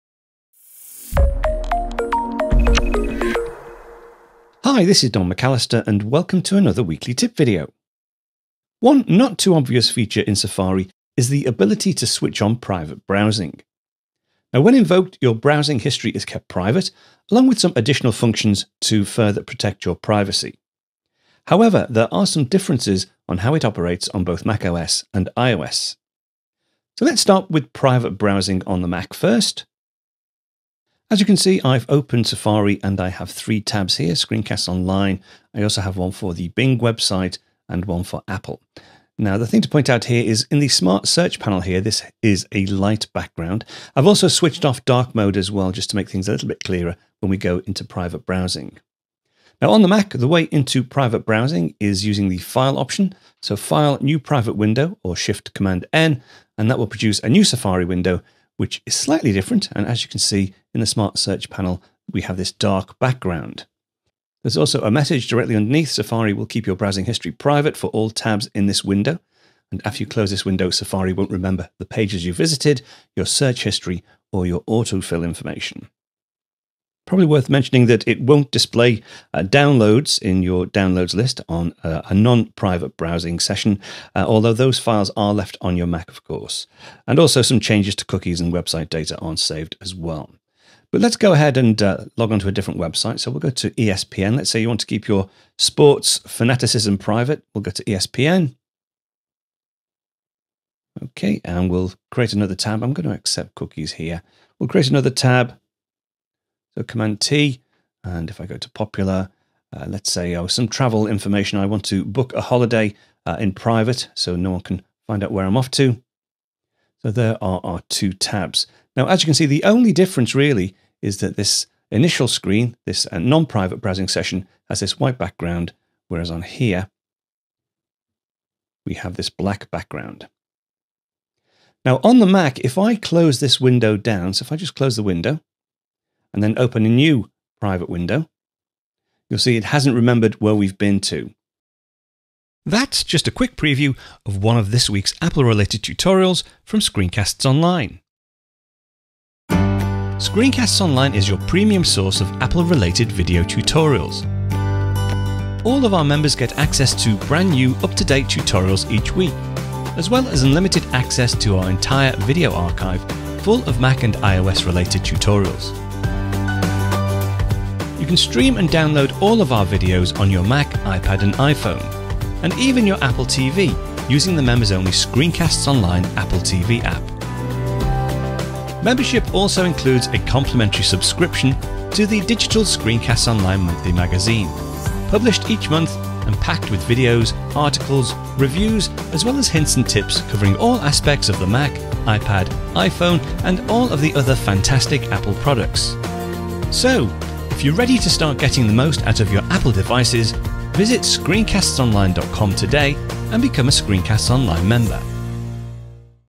SCOM0962 - Tip - Safari Private Browsing - Preview
This is a short preview of a full ScreenCastsOnline video tutorial. for a free 7-day membership. - A little-known feature of Safari on both macOS and iOS is the ability to browse in private. This allows you to keep your browsing history private as well as take advantage of some additional privacy features.

In this tip video, Don takes you through the standard usage of Private Browsing in Safari, on both the Mac and iPad, as well as pointing out issues to be aware of on iOS.